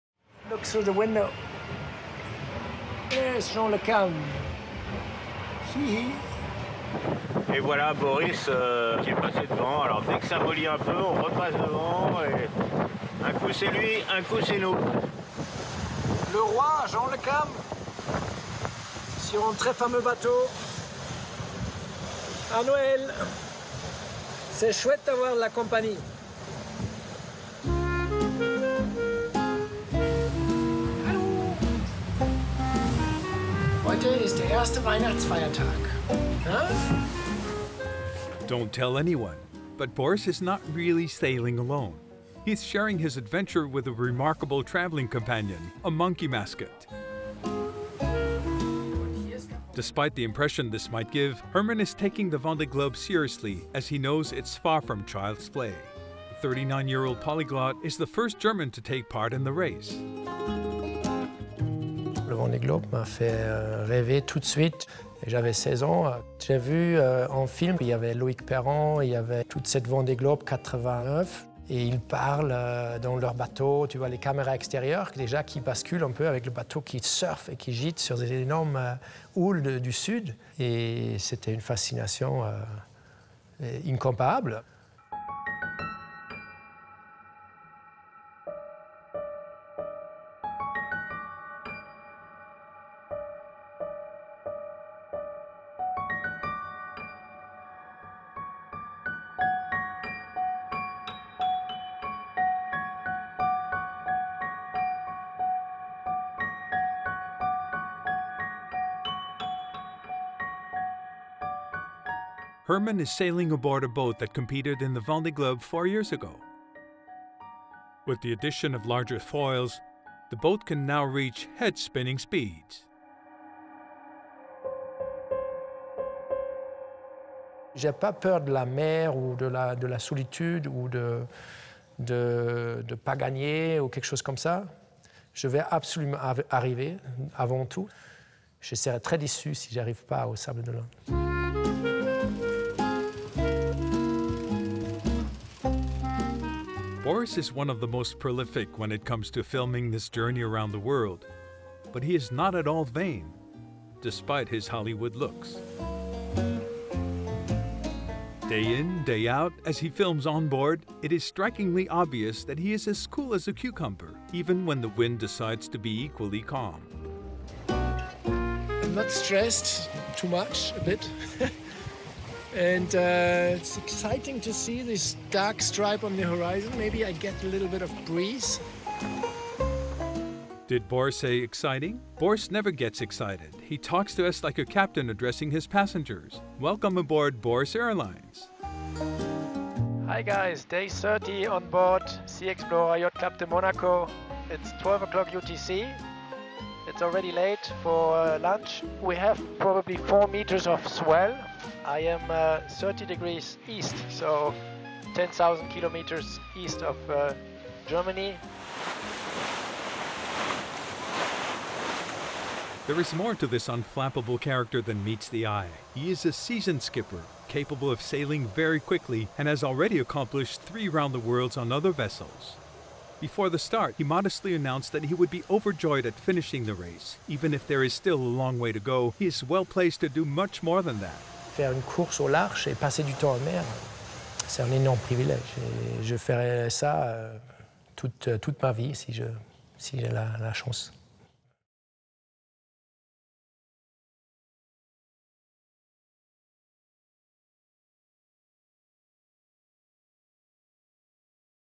Sailing's Great Entertainer | Boris Herrmann | Vendée Globe
His engaging video updates have made German Boris Herrmann one of the breakout stars of the 2020 Vendée Globe. We meet the man destined for a life at sea